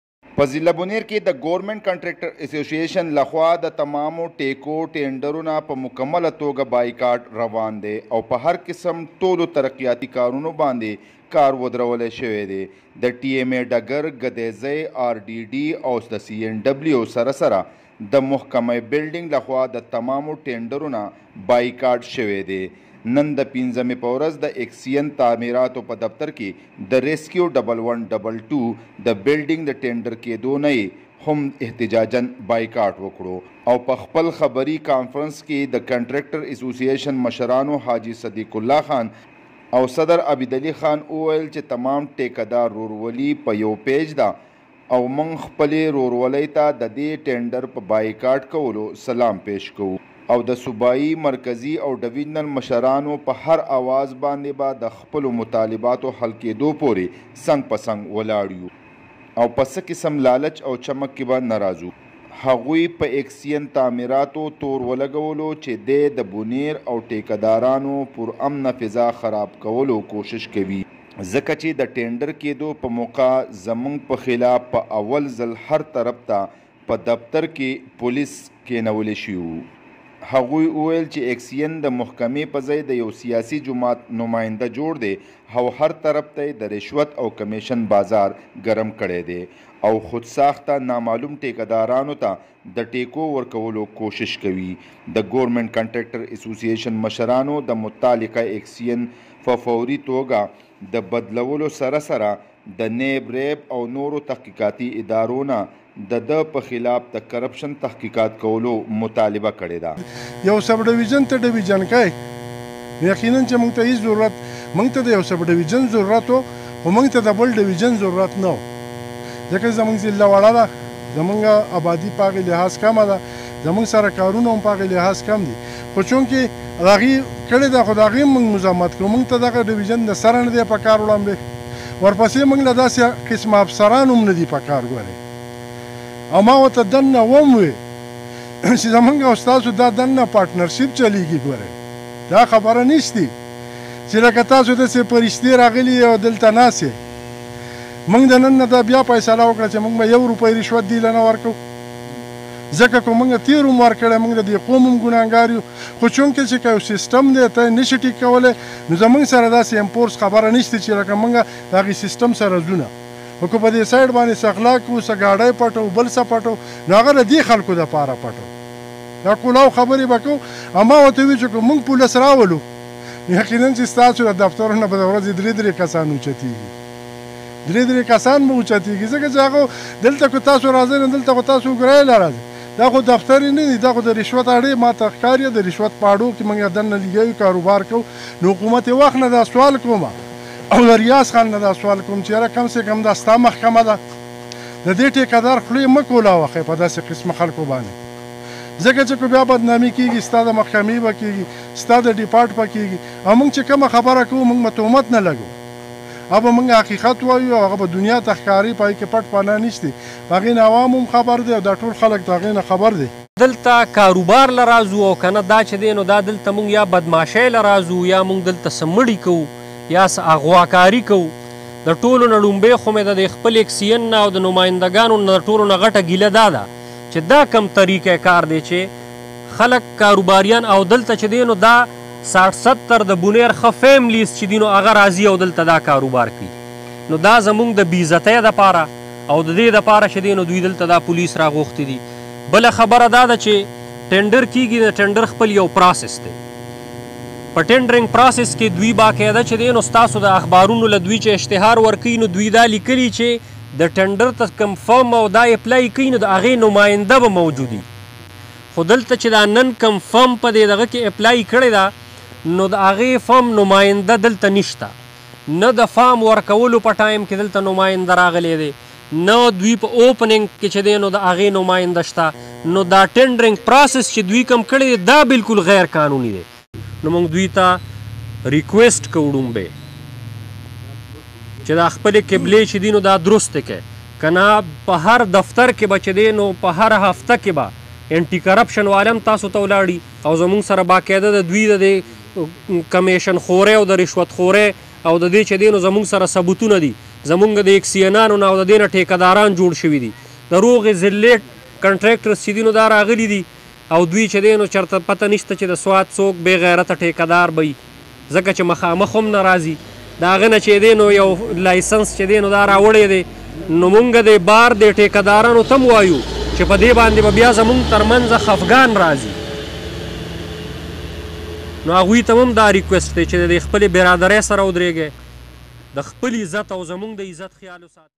بونیر: گورنمنٹ کنٹریکٹر ایسوسی ایشن کا احتجاج جاری محکمہ ایکسیئن بلڈنگ کے ٹینڈر بھی بائی کاٹ
بونیر: گورنمنٹ کنٹریکٹر ایسوسی ایشن کا احتجاج جاری محکمہ ایکسیئن بلڈنگ کے ٹینڈر بھی بائی کاٹ کے موقع پر چیئرمین حاجی صدیق اللہ اور صدر عابد علی خان کا میڈیا سے گفتگو۔